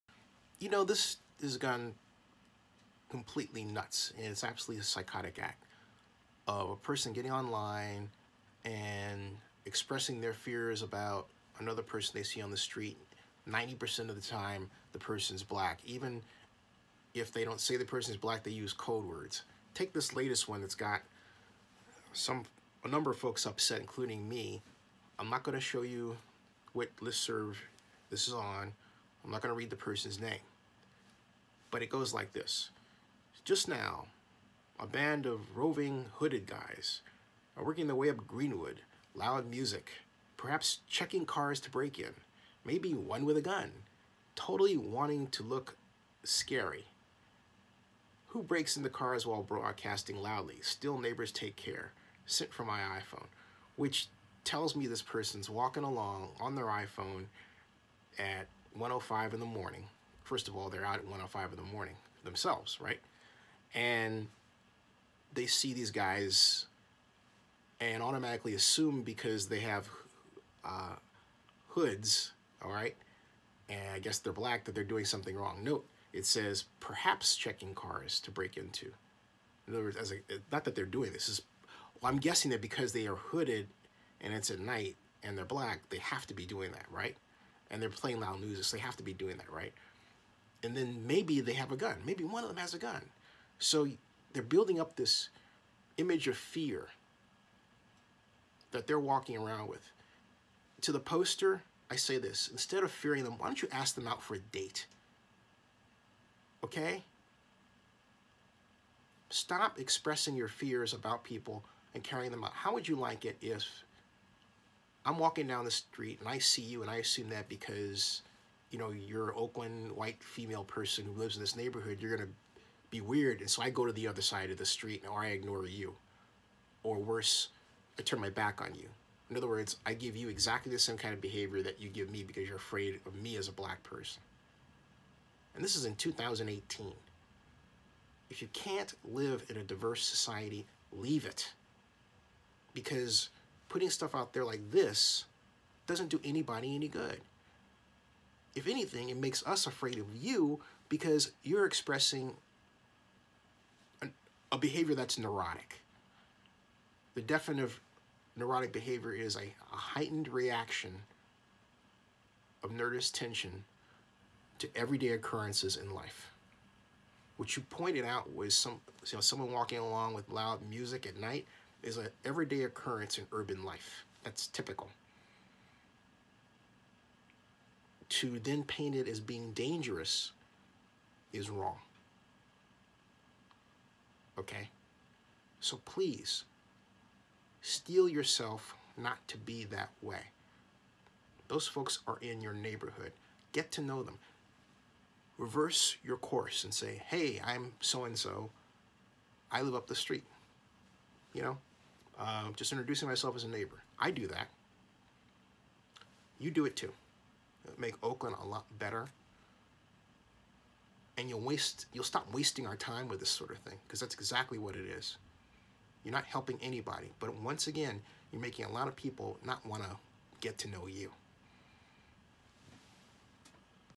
Can Oakland See A Day Without Online Racial Profiling? A Vlog Message To The Latest Evil-Doer
Zennie62 Can Oakland See A Day Without Online Racial Profiling? A Vlog Message To The Latest Evil-Doer オークランドの襲撃者 - (Zennie62 YouTube vlog) Maria Ayerdi Kaplan